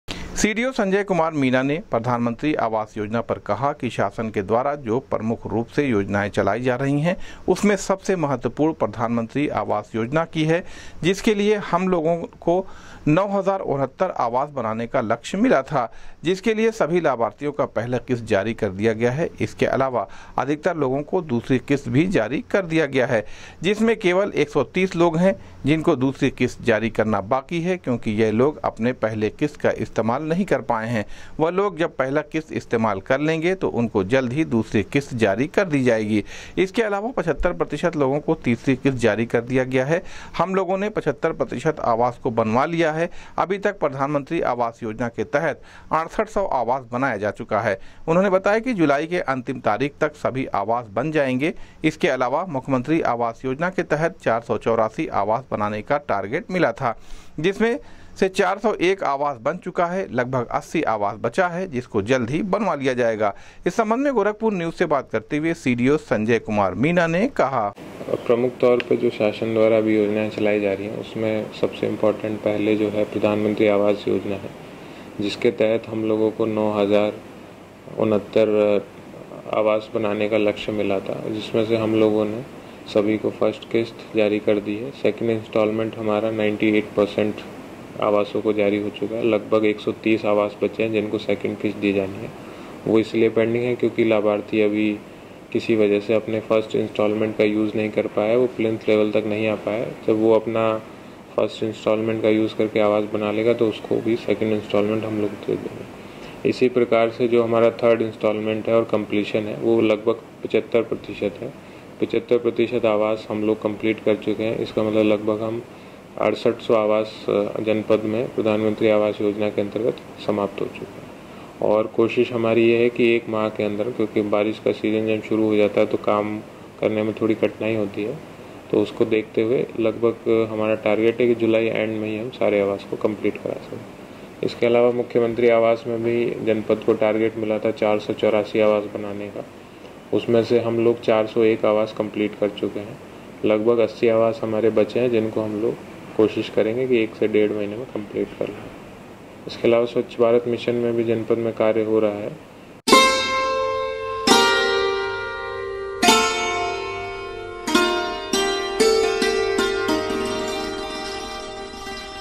जनपद में अभी तक ग्रामीण क्षेत्रो में प्रधानमंत्री आवास योजना के तहत 6800 व मुख्यमंत्री आवास
जनपद में अभी तक ग्रामीण क्षेत्रो में प्रधानमंत्री आवास योजना के तहत 6800 व मुख्यमंत्री आवास योजना के तहत अभी तक 401 बनाया जा चूका है आवास,सीडीओ संजय कुमार मीना ने दी जानकारी